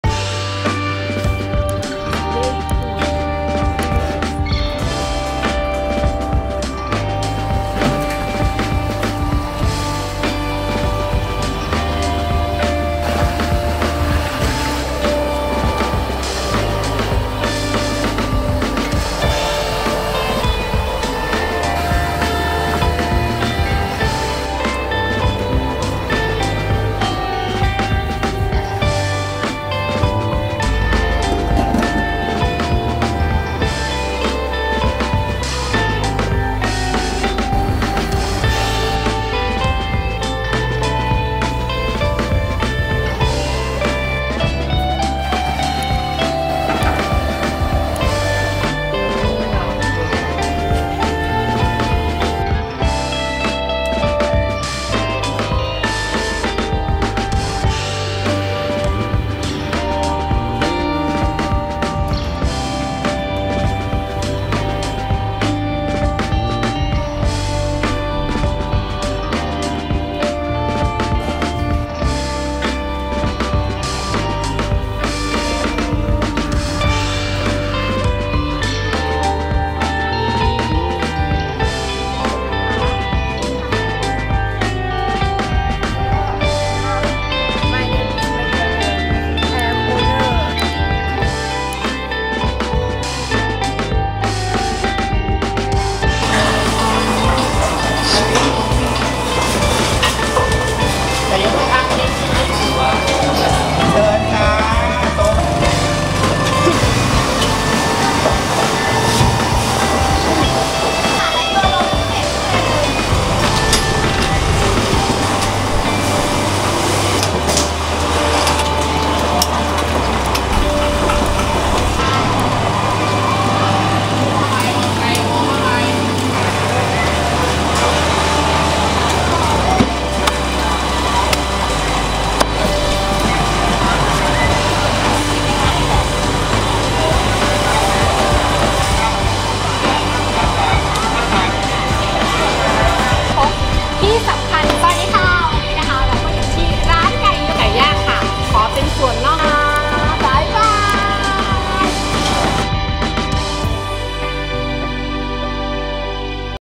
BEEF AND PORK SPECIAL SOUP - STREET FOOD PATTAYA THAILAND - SERVE BY MESMERIZING GIRL
🌷 Beef and pork soup served by mesmerizing girl pattaya thailand.

Thanks.
You're Mesmerizing.